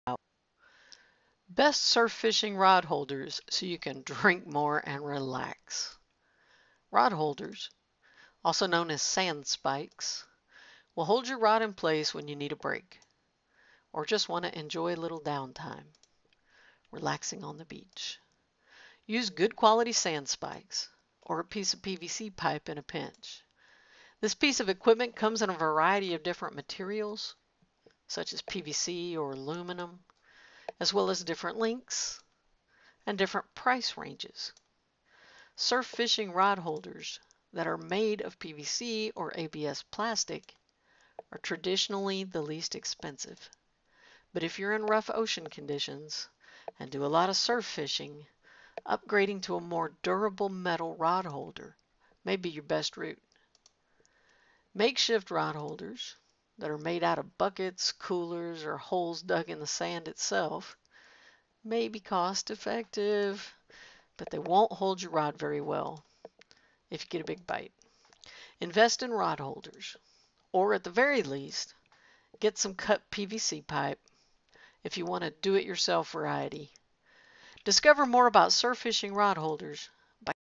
Best Surf Fishing Rod Holders So You Can Drink More And Relax
surf fishing rod holders

Rod holders, also known as Sand Spikes, will hold your rod in place when you need a break, or just want to enjoy a little downtime relaxing on the beach. Use good quality sand spikes or a piece of PVC pipe in a pinch.

This piece of equipment comes in a variety of different materials – such as PVC or aluminum – as well as different lengths, and different price ranges. Rod holders that are made of PVC or ABS plastic are traditionally the least expensive, but if you’re in rough ocean conditions and do a lot of surf fishing, upgrading to a more durable metal rod holder may be your best route.

Makeshift rod holders that are made out of buckets, coolers, or holes dug in the sand itself may be cost effective, but they won’t hold your rod very well if you get a big bite. Invest in rod holders, or at the very least, get some cut PVC pipe if you want a do-it-yourself variety. Best Surf Fishing Rod Holders So You Can Drink More And Relax